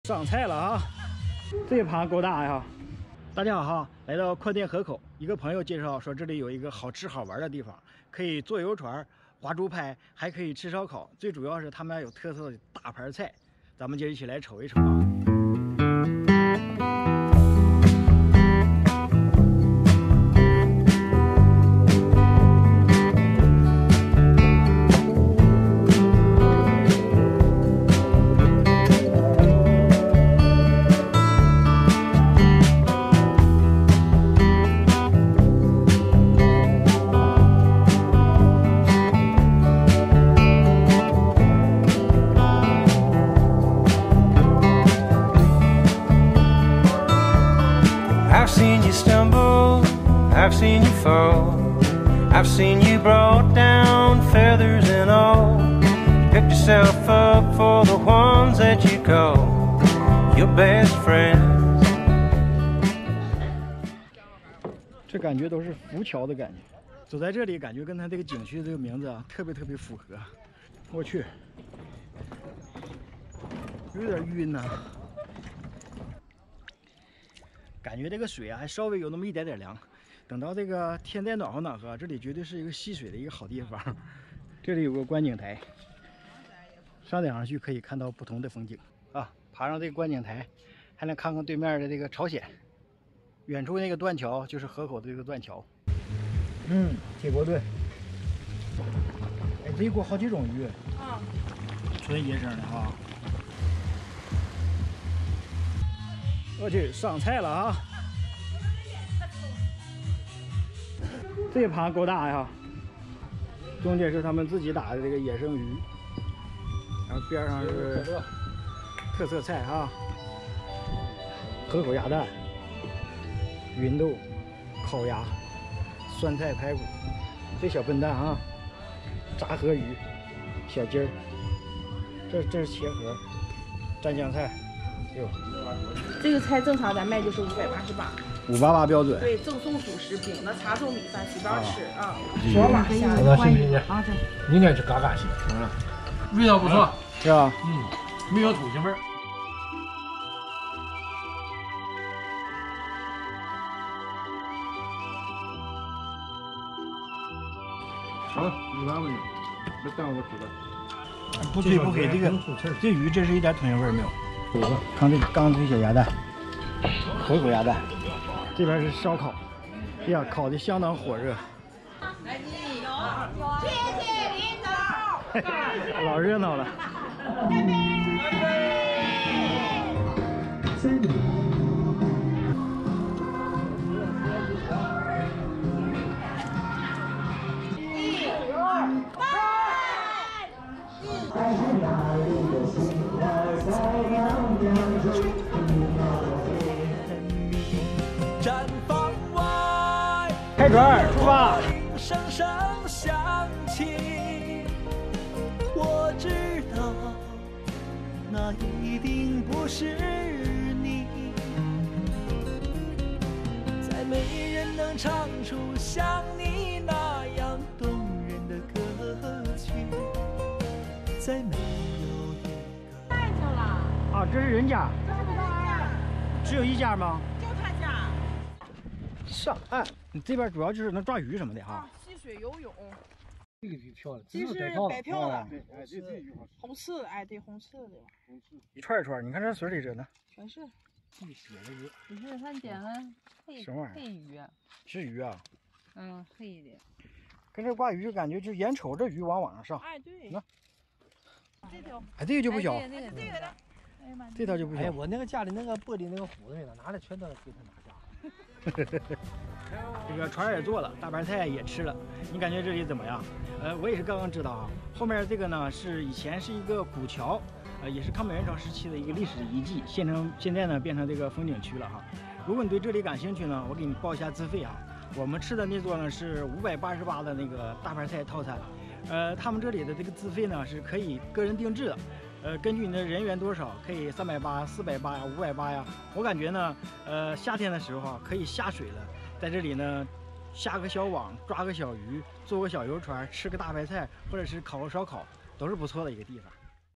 丹东河口这生态园真好，坐游船看朝鲜吃烧烤，十个人一盘菜就够了
【丹东小宇哥】丹东河口这生态园真好，坐游船看朝鲜吃烧烤，十个人一盘菜就够了

家乡丹东是全国最大的边境城市。歌唱家乡！
【丹东小宇哥】原创系列 未经许可 不得转载